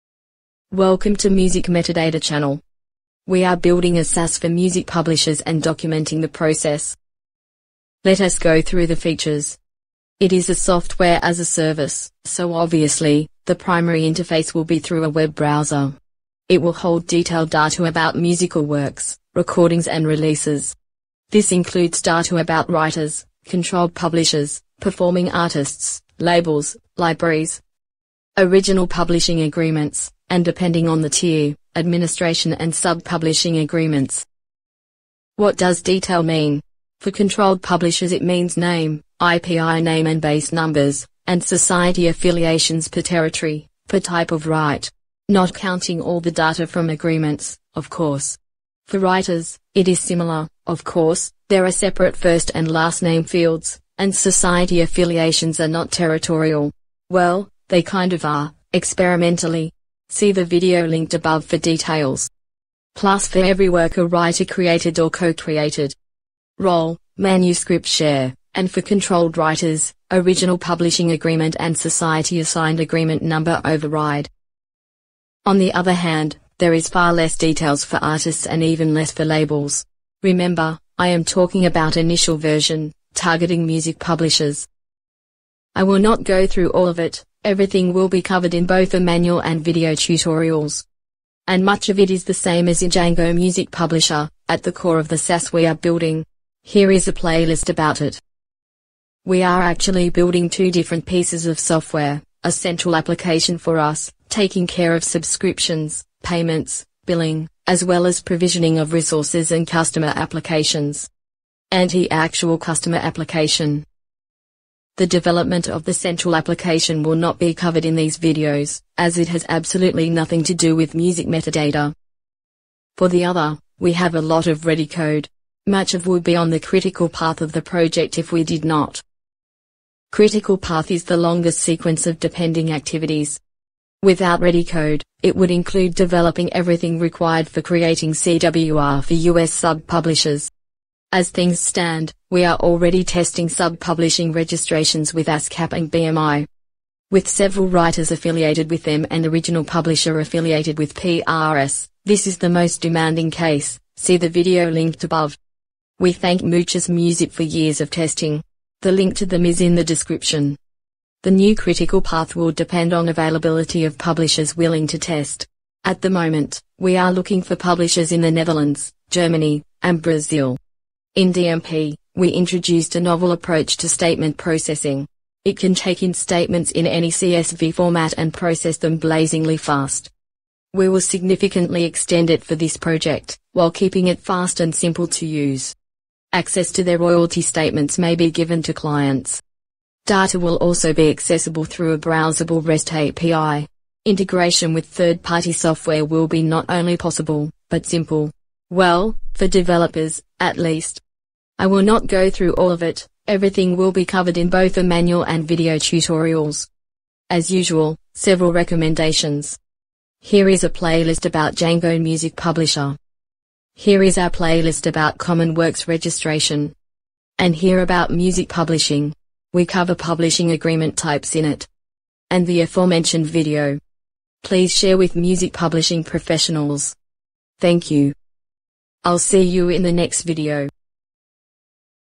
Building Software as a Service for Music Publishers (Music Publishing Administration)
We are building software as a service for music publisher and documenting the whole process.

In this video, we give a feature overview.